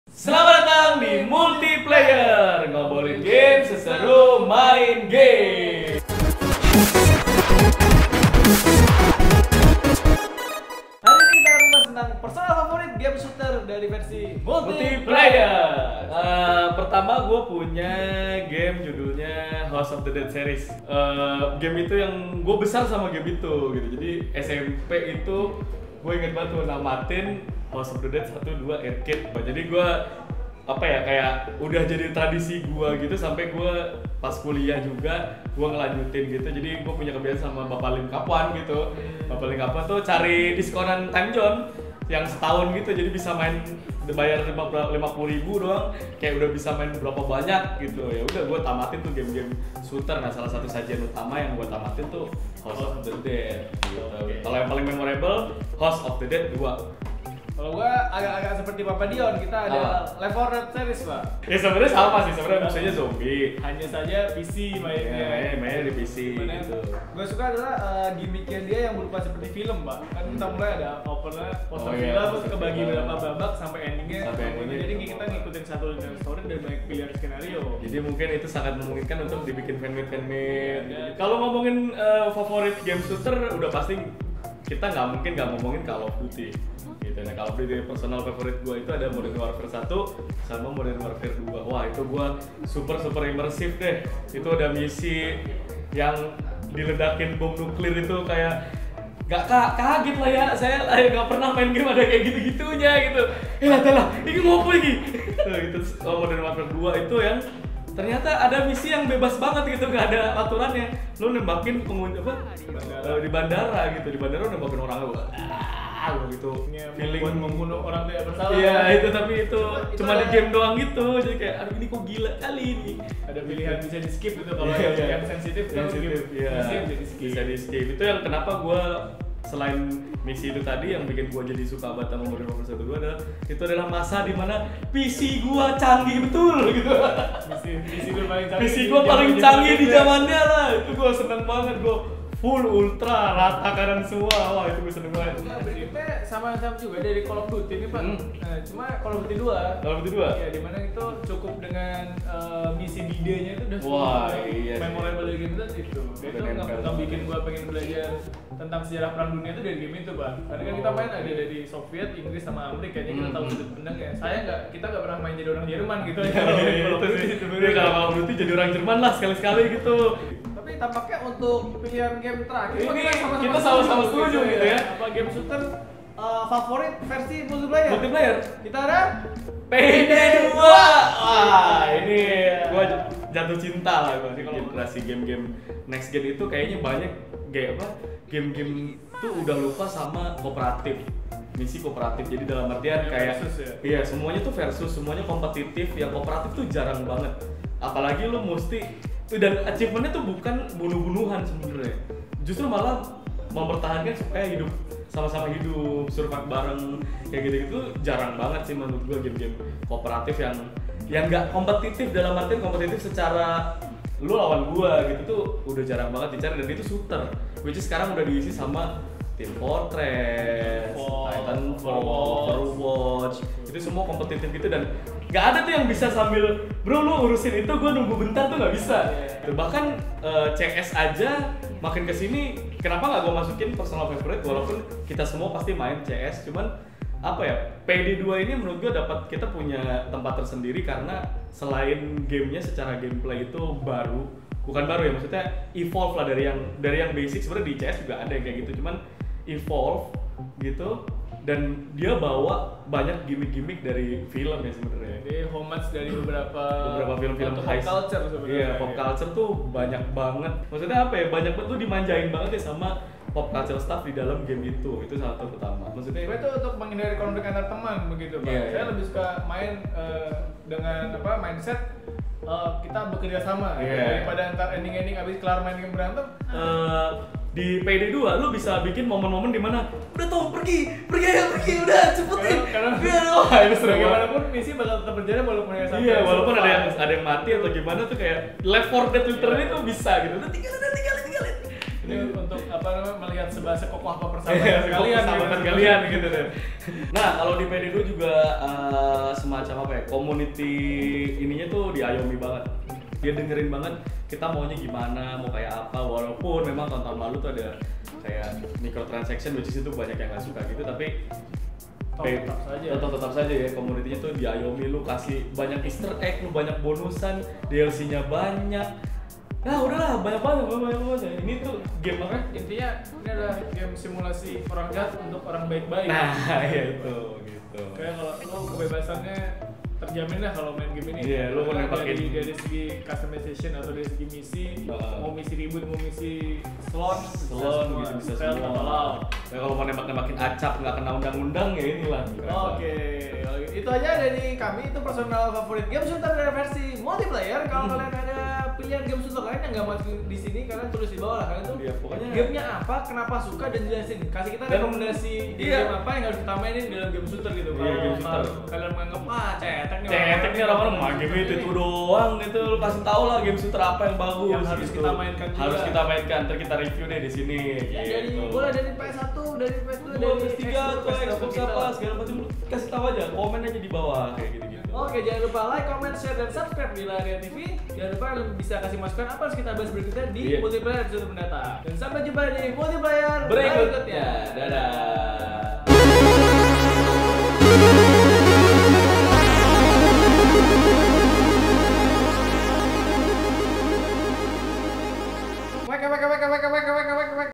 Game Shooter Terbaik - Multiplayer Episode 7
Kali ini Dion dan Tomi bakal ngobrolin soal game shooter favorit versi Multiplayer. Game shooter favorit kamu sama kayak Dion dan Tomi gak? Kalo engga, kamu bisa sebutin game shooter versi kamu di kolom komentar ya.

Nantikan juga episode lainnya setiap hari Selasa jam 7 malam, karena Multiplayer "Ngomongin game, seseru main game!"